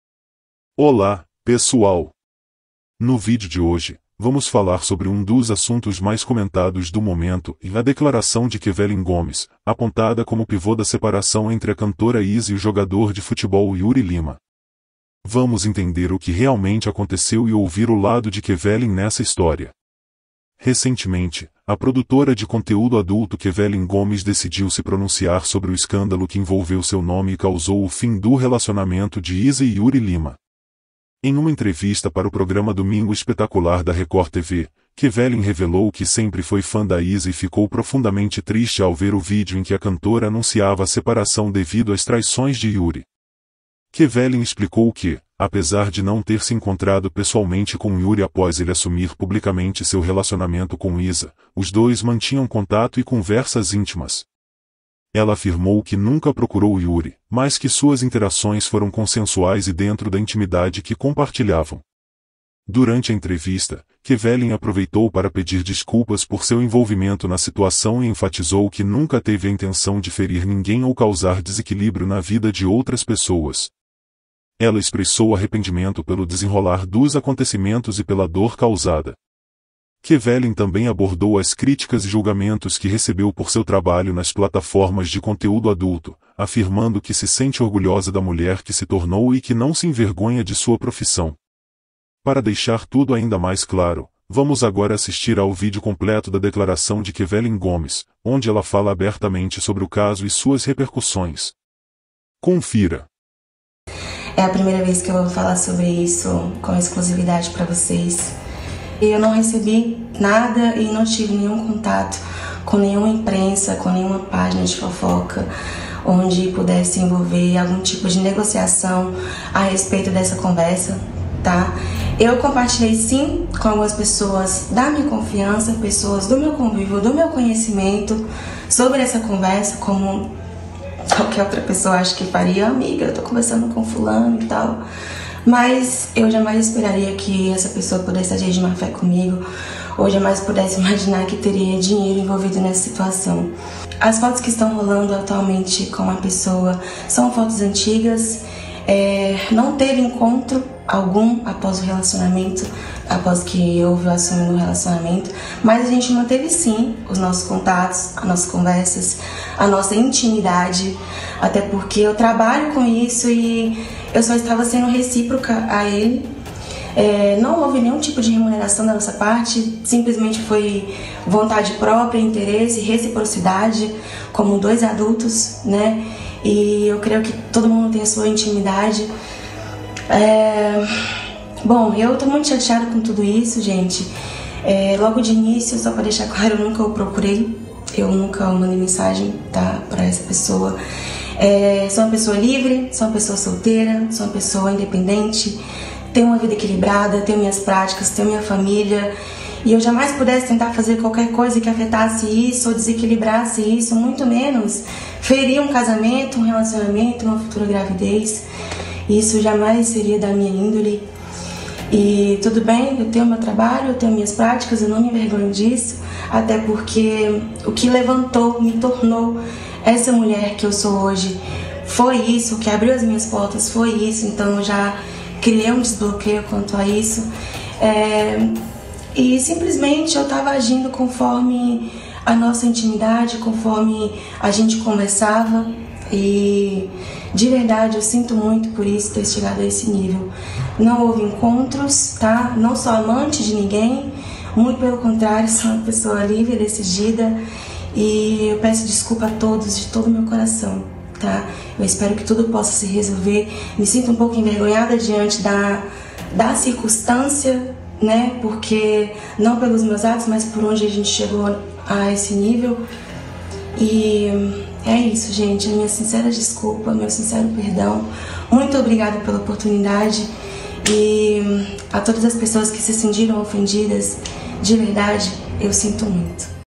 Kevelin Gomes Quebra o Silêncio Sobre a Polêmica com Yuri Lima e Iza
Este vídeo aborda a declaração de Kevelin Gomes, envolvida no escândalo que resultou na separação da cantora Iza e do jogador de futebol Yuri Lima. Kevelin, produtora de conteúdo adulto, esclarece sua relação com Yuri, afirmando que eles mantinham contato e conversas íntimas, mas sem encontros pessoais após ele ter assumido o relacionamento com Iza. Ela expressa tristeza por ter sido um fator na separação e pede desculpas, destacando que nunca teve a intenção de causar danos. Kevelin também comenta sobre os julgamentos que recebeu por sua profissão e reafirma seu orgulho pela carreira que construiu. O vídeo inclui a íntegra da declaração de Kevelin sobre o caso.